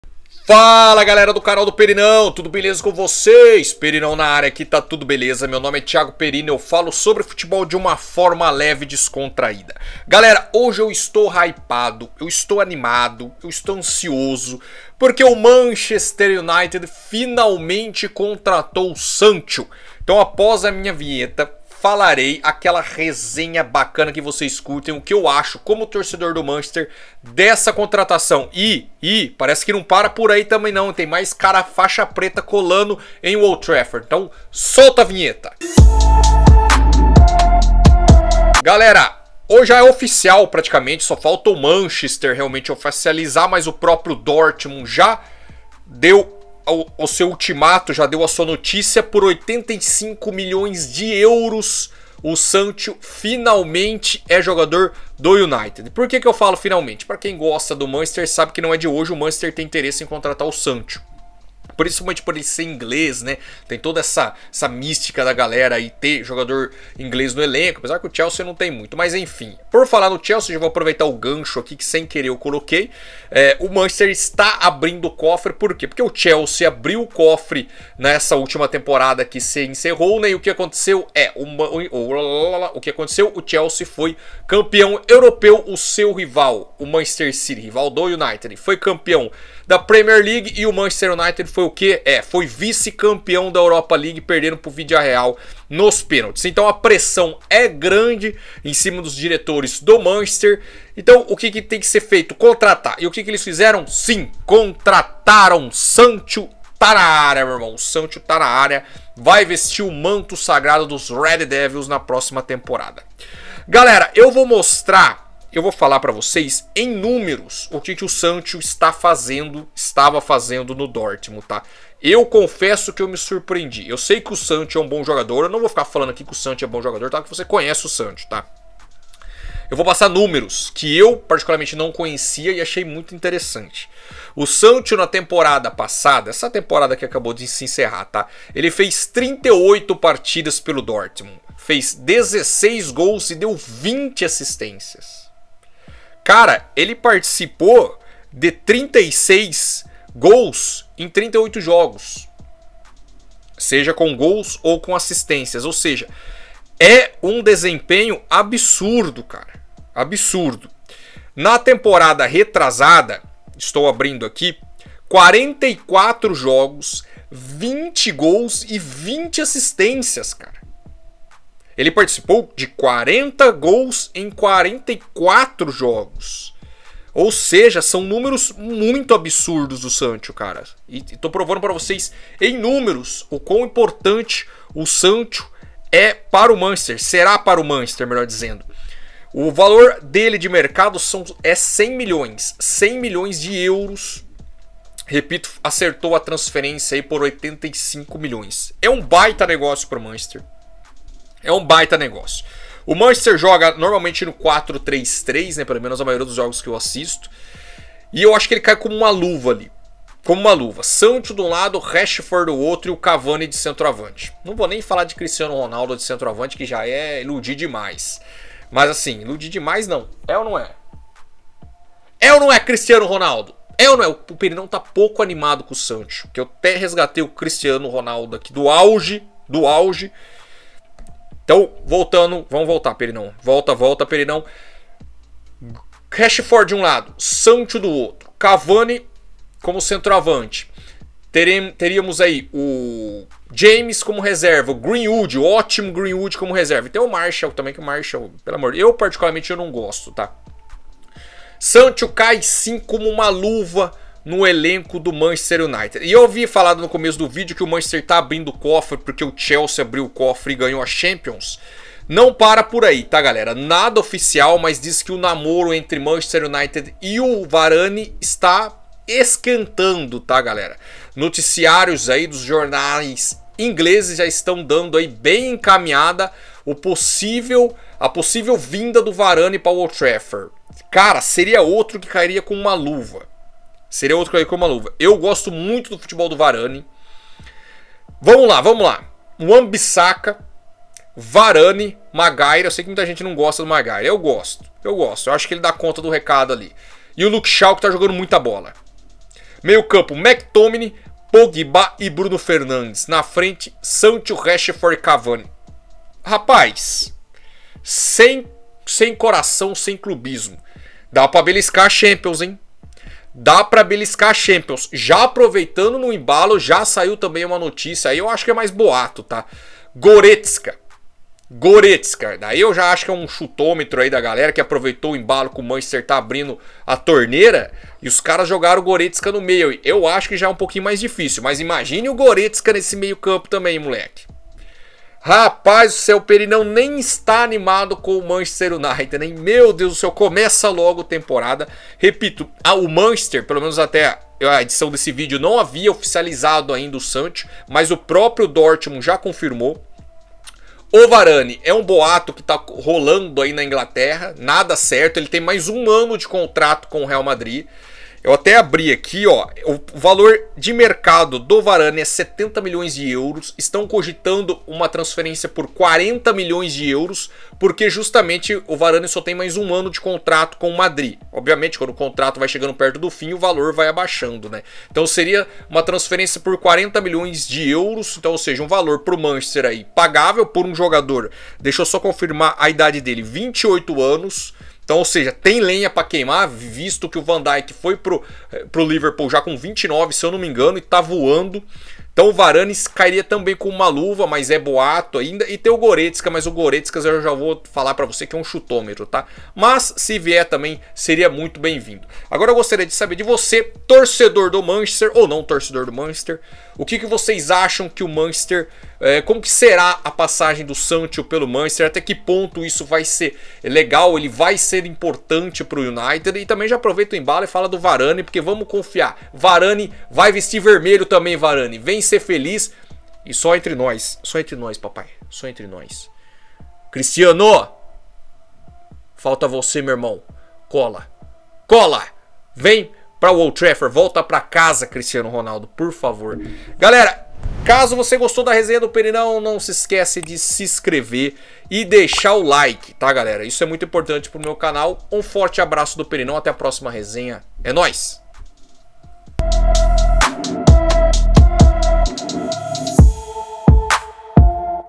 SANCHO, VARANE, GORETZKA...DESSA VEZ VAI MANCHESTER UNITED?!
Salve amantes do futebol, beleza?

Bora pra resenha sobre os possíveis reforços do United pra essa temporada.

Se tiver sugestões deixe nos comentários!

Obrigado pela paciência, talvez piore com o tempo :x